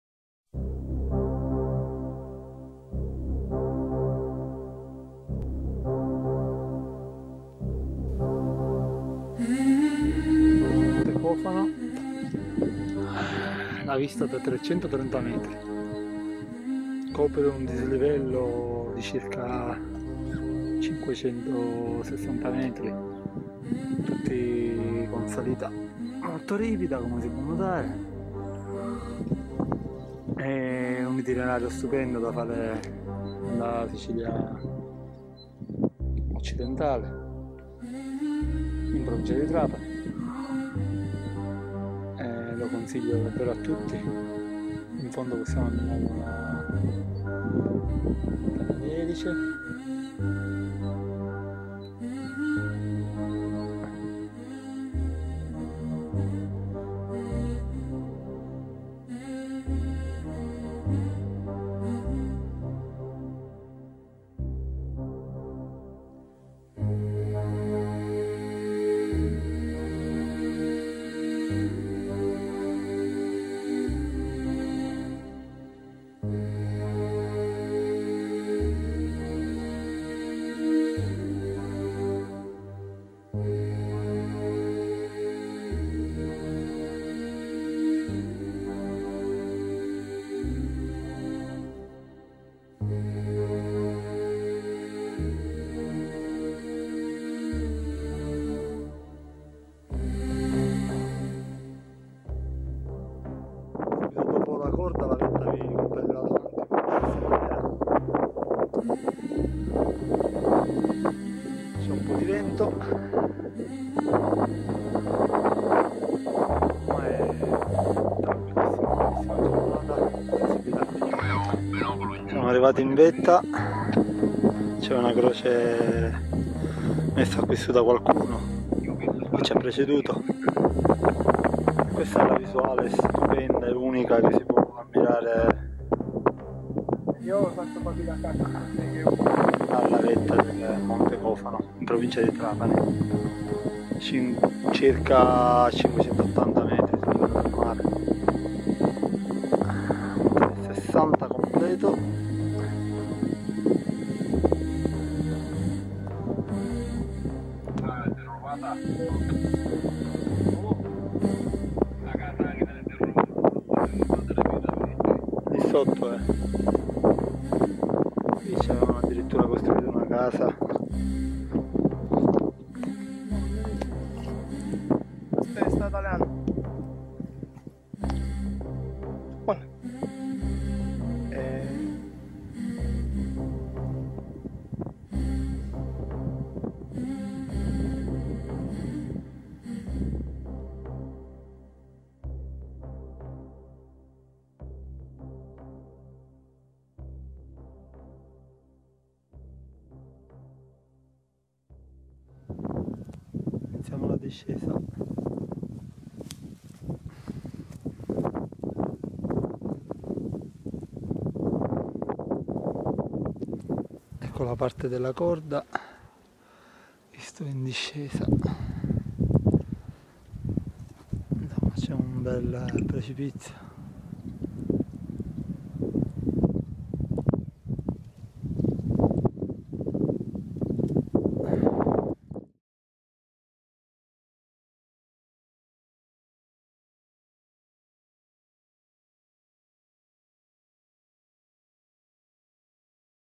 Monte Cofano la vetta..
Scalata del monte Cofano 659m a Custonaci (tp), in data 22 maggio 2014.
Itinerario stupendo e non troppo difficile, lo consiglio a tutti gli amanti della Natura e dei panorami mozzafiato.